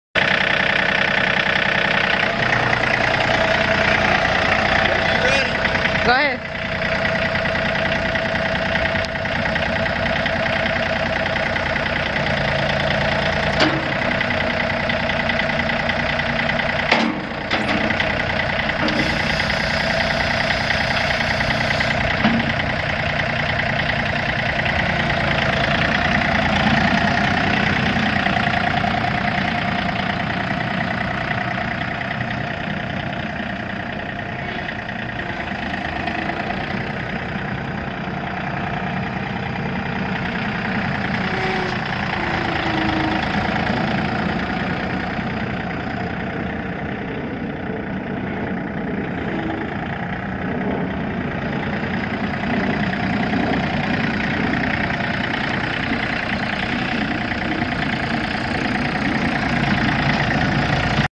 2017 John Deere 3025E MFWD tractor for sale at auction | bidding closes February 27, 2019
2017 John Deere 3025E MFWD tractor
3 hours on meter
Yanmar 3TNV88F-EPJT4 1.64L three cylinder diesel engine
19.4 HP
Serial CH3N17D006587
Hydrostatic transmission
Differential lock
Folding ROPS
Tilt steering wheel
Suspension seat
John Deere D16 loader
62"W bucket
Joystick controls
Three point
63" wheelbase
25x8.50-14 NHS front tires
15-19.5 NHS R-4 rear tires
Serial 1LV3025ETHH107609
It's located in Fort Worth, TX, and we're selling it to the highest bidder regardless of price.